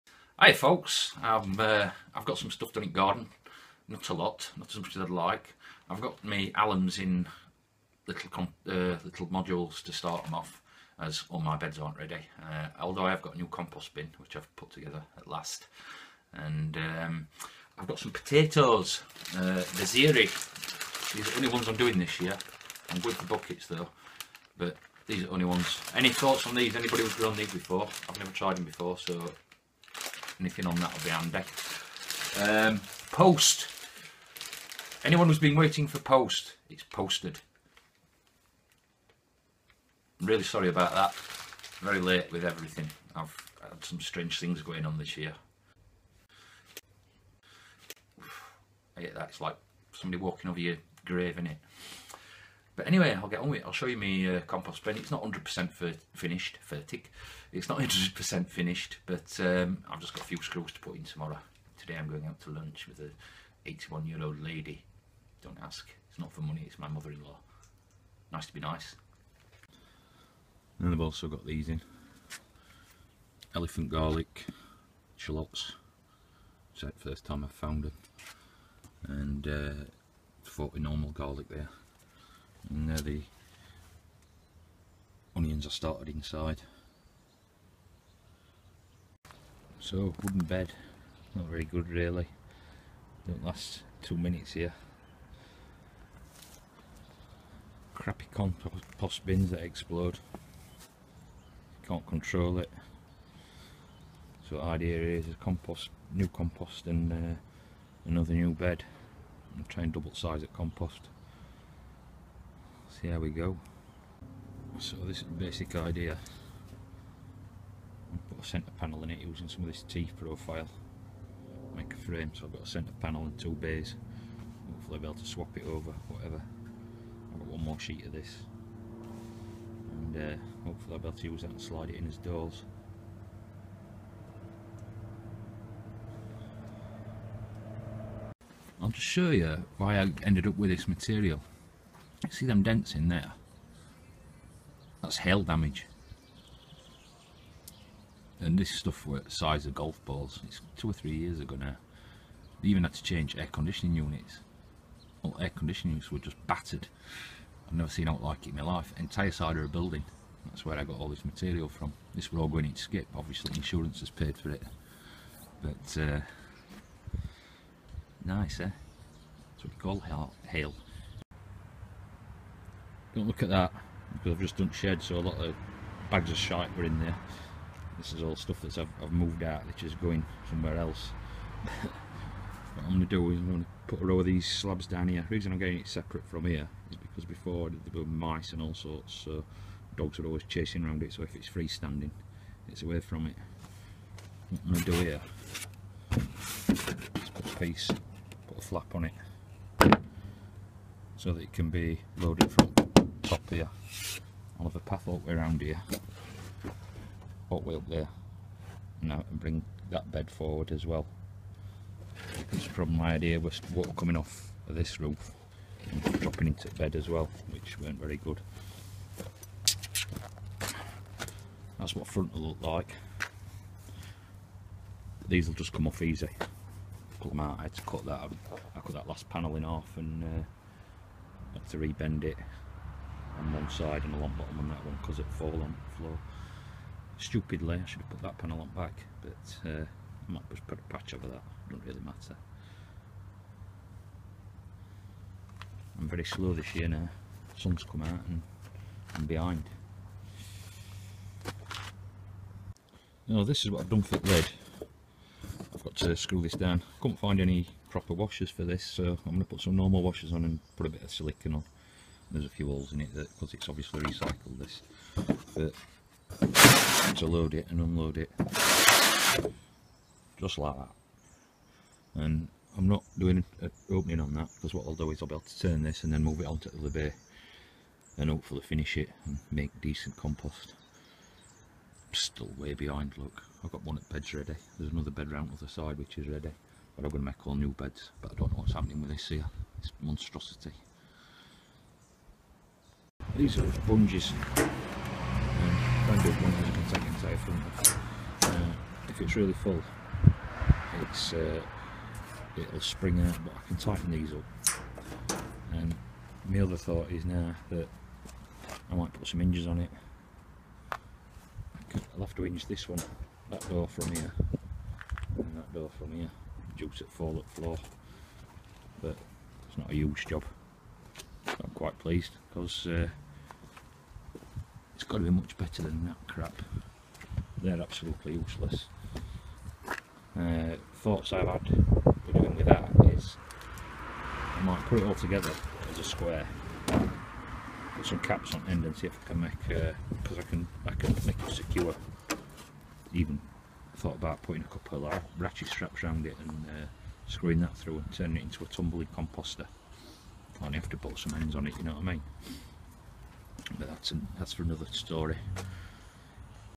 New compost bins ; 13 03 16
New compost bin Desiree potatoes Elephant garlic. We have a start on the garden ;-)